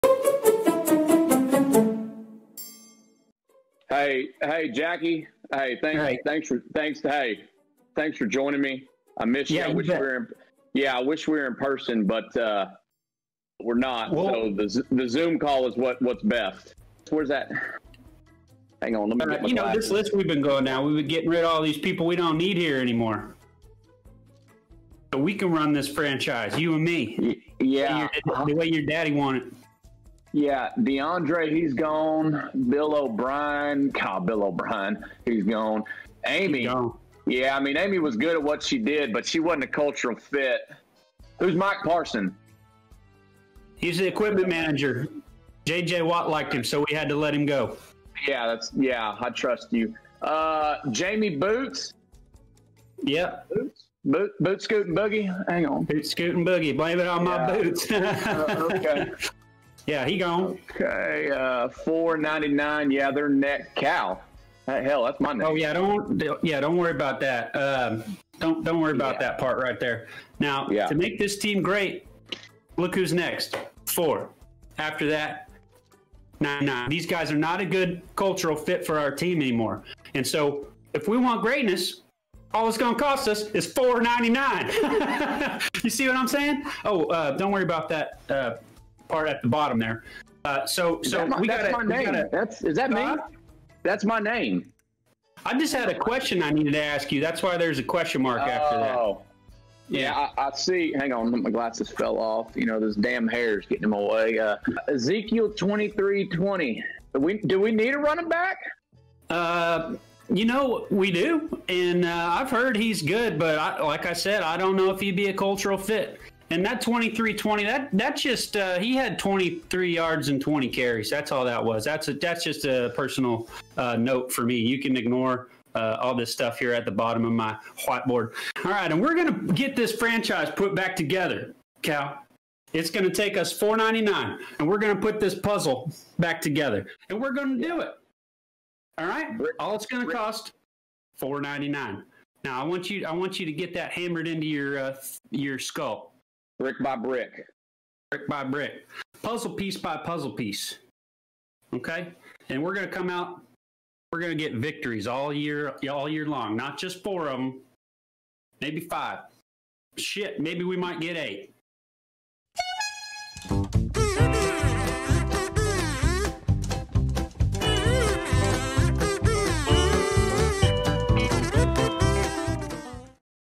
Cal & Jack - Whhhiteboard
Cal & Jack discuss their plan for the Texans over Zoom